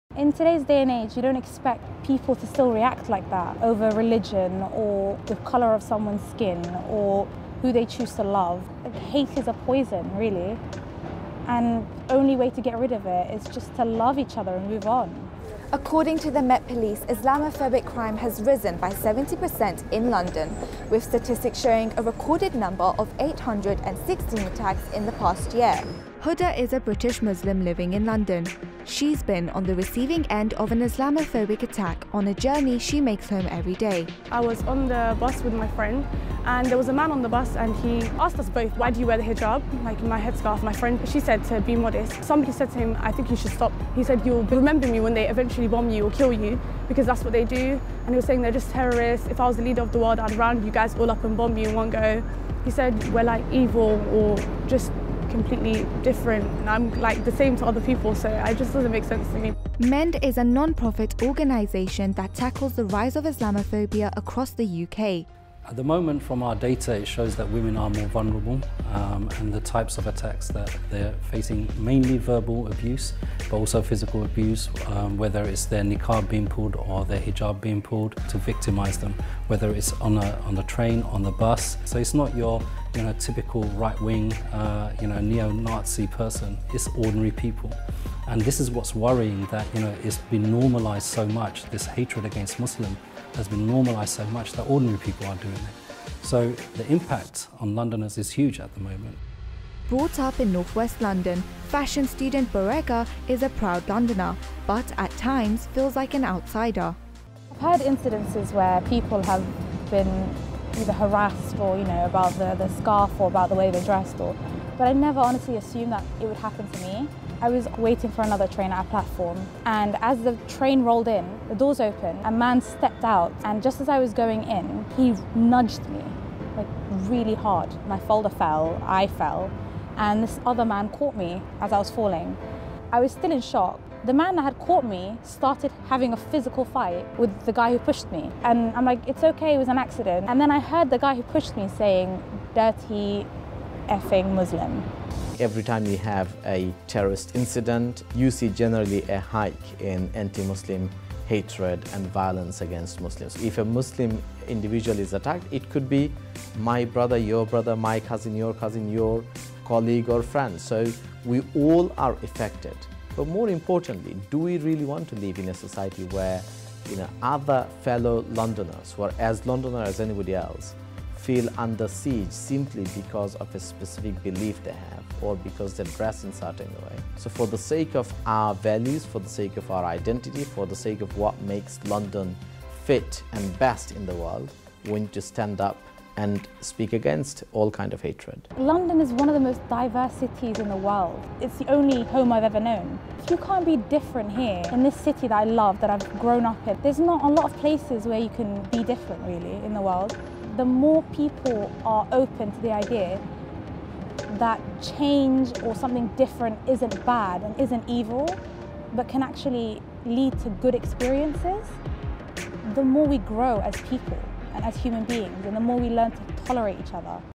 Feature: Islamophobia
ISLAMOPHOBIA
According to Tell MAMA, an organisation that supports victims of anti-Muslim hate, 60% of Londoners who have experienced Islamaphobia in the last year were women. Farhat Saede has the story.

Stay updated with what goes on behind the scenes!